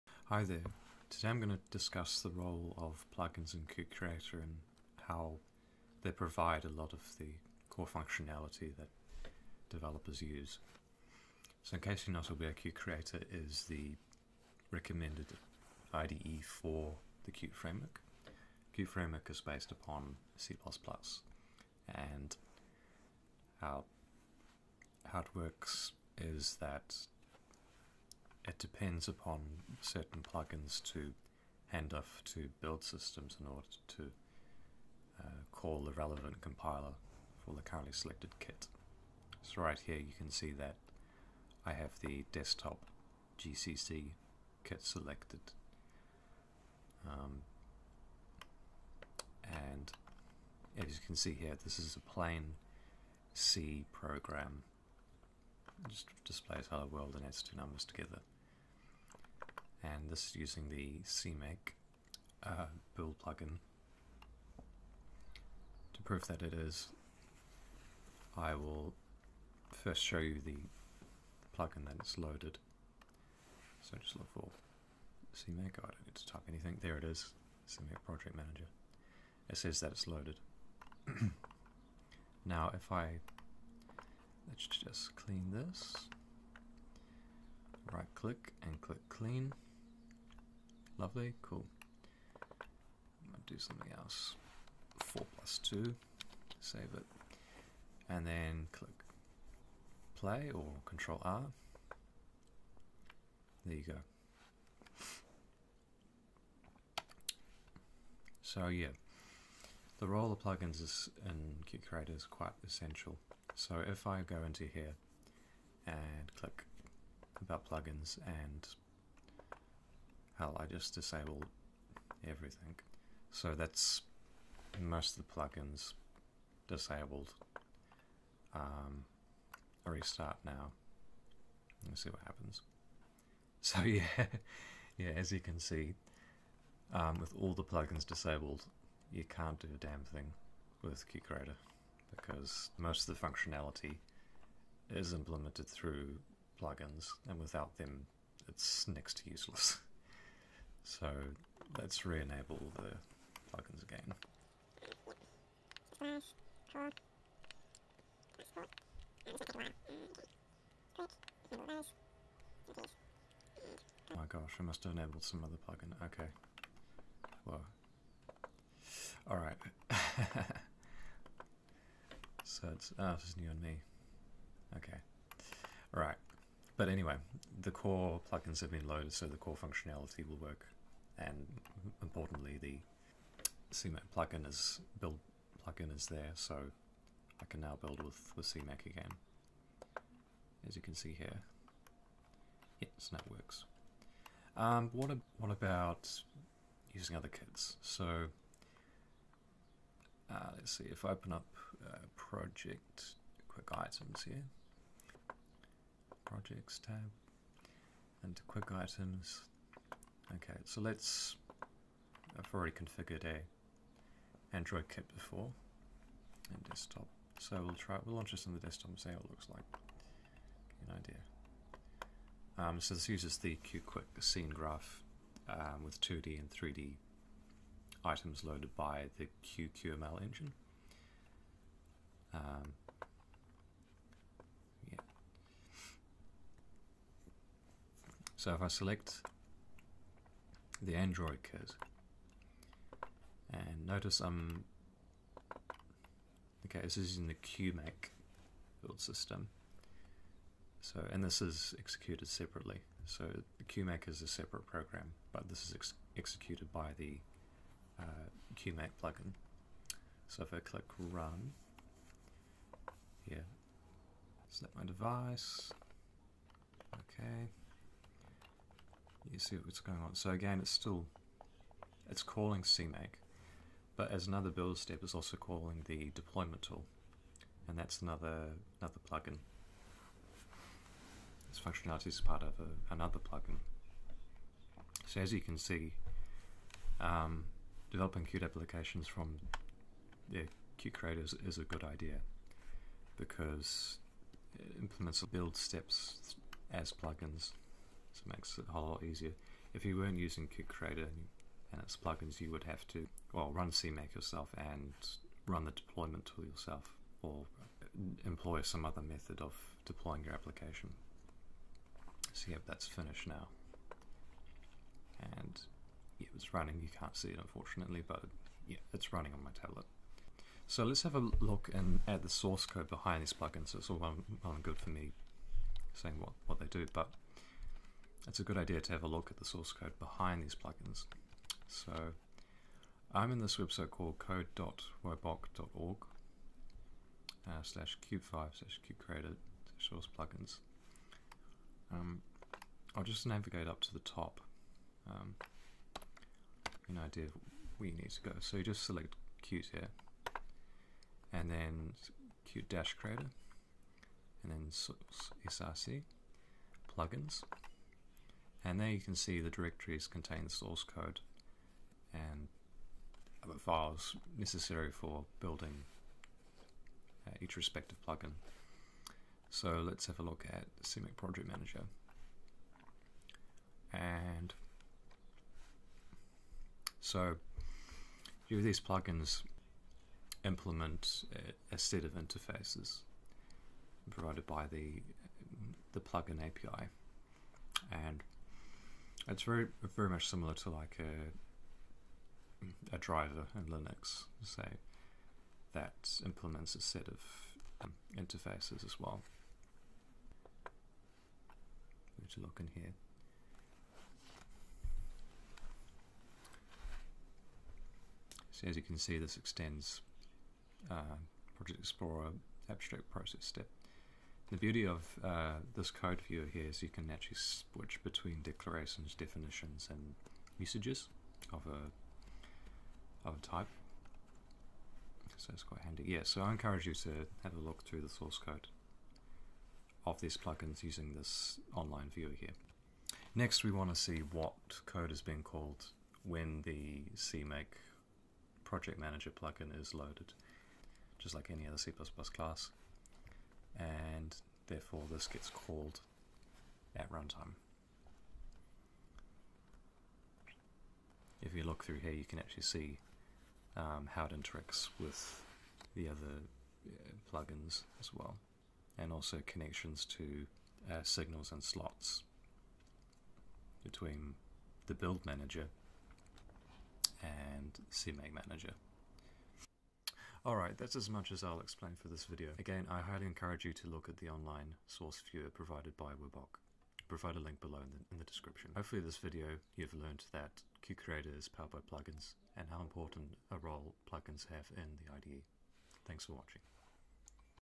Qt Creator: Everything's a Plugin
I explain how Qt Creator relies on plugins for most of its functionality, and talk about build plugins. Browse the code for the plugins using woboq's online viewer